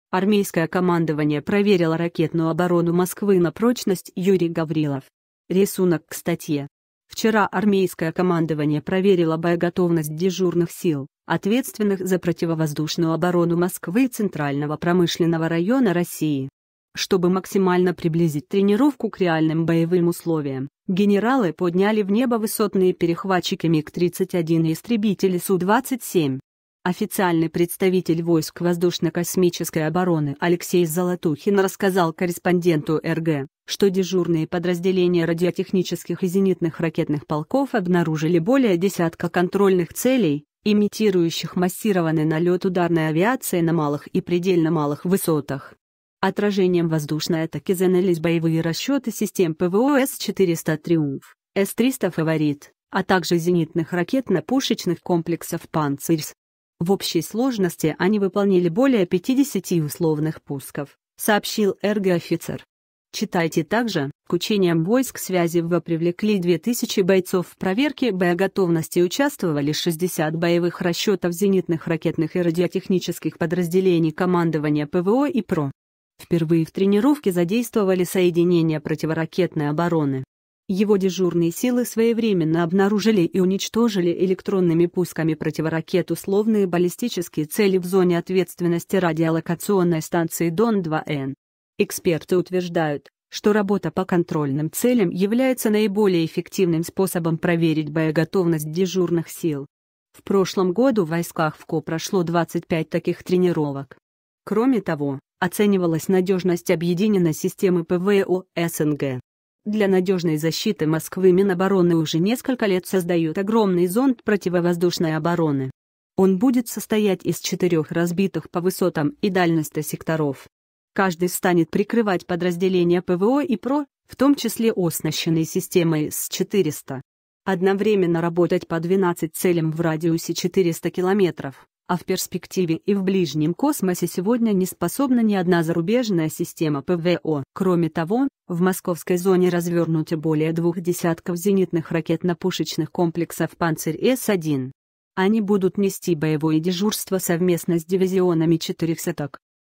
RG150128 010 Армейское командование проверило ракетную обо
Армейское командование проверило ракетную оборону Москвы на прочность Юрий Гаврилов Вчера армейское командование проверило боеготовность дежурных сил, ответственных за противовоздушную оборону Москвы и Центрального промышленного района России. Чтобы максимально приблизить тренировку к реальным боевым условиям, генералы подняли в небо высотные перехватчики МиГ-31 и истребители Су-27. Официальный представитель Войск воздушно-космической обороны Алексей Золотухин рассказал корреспонденту "РГ", что дежурные подразделения радиотехнических и зенитных ракетных полков обнаружили более десятка контрольных целей, имитирующих массированный налет ударной авиации на малых и предельно малых высотах. - Отражением воздушной атаки занялись боевые расчеты систем ПВО С-400 "Триумф", С-300 "Фаворит", а также зенитных ракетно-пушечных комплексов "Панцирь-С". В общей сложности они выполнили более 50 условных пусков, - сообщил "РГ" офицер. читайте также К учениям войск связи ВВО привлекли две тысячи бойцов В проверке боеготовности участвовали 60 боевых расчетов зенитных ракетных и радиотехнических подразделений командования ПВО и ПРО. Впервые в тренировке задействовали соединение противоракетной обороны. Его дежурные силы своевременно обнаружили и уничтожили электронными пусками противоракет условные баллистические цели в зоне ответственности радиолокационной станции "Дон-2Н". Эксперты утверждают, что работа по контрольным целям является наиболее эффективным способом проверить боеготовность дежурных сил. В прошлом году в Войсках ВКО прошло 25 таких тренировок. Кроме того, оценивалась надежность Объединенной системы ПВО СНГ. Для надежной защиты Москвы Минобороны уже несколько лет создает огромный зонт противовоздушной обороны. Он будет состоять из четырех разбитых по высотам и дальности секторов. Каждый станет прикрывать подразделения ПВО и ПРО, в том числе оснащенные системой С-400. Одновременно работать по 12 целям в радиусе 400 километров, а в перспективе и в ближнем космосе сегодня не способна ни одна зарубежная система ПВО. Кроме того, в московской зоне развернуты более двух десятков зенитных ракетно-пушечных комплексов "Панцирь-С1". Они будут нести боевое дежурство совместно с дивизионами "четырехсоток".